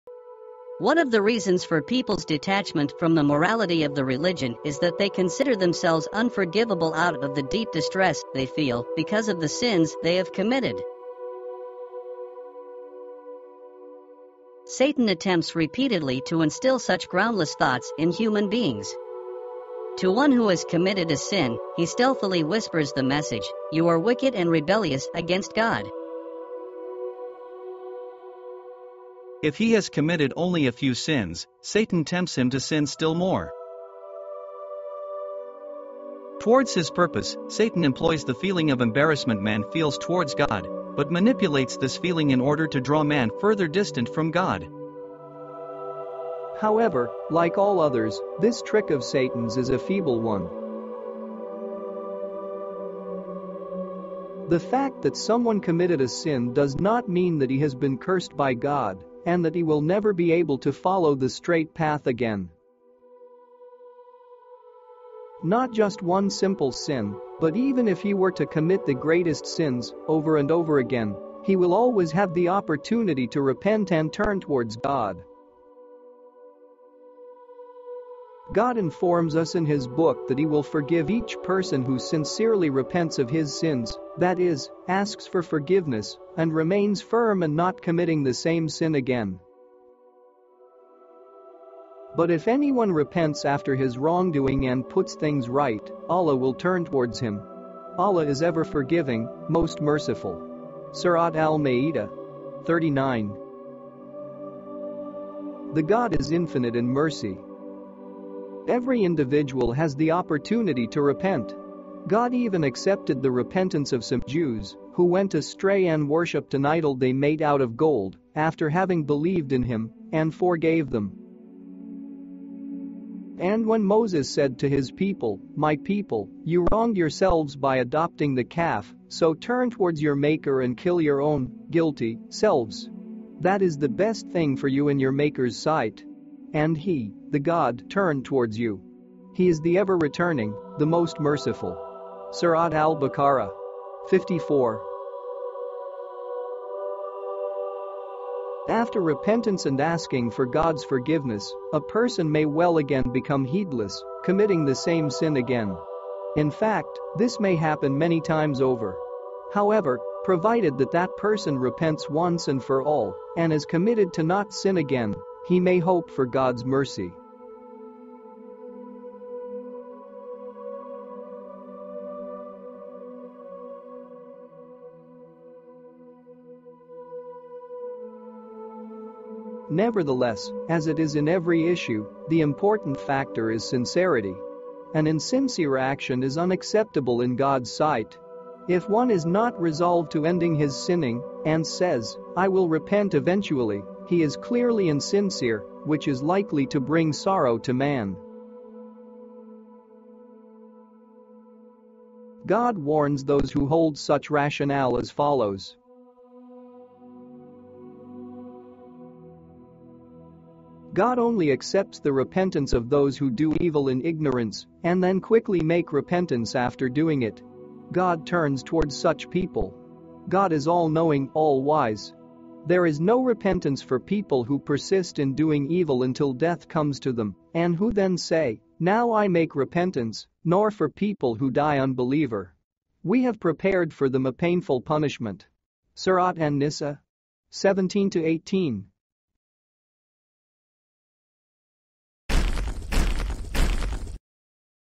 Why Satan MANIPULATES your FEELINGS To Keep You Away From GOD
I always had a strong sense of feeling uneasy and unsettled in my day-to-day life. Demons and evil spirits have very fearful and imaginative ways of twisting your thoughts to keep you away from God.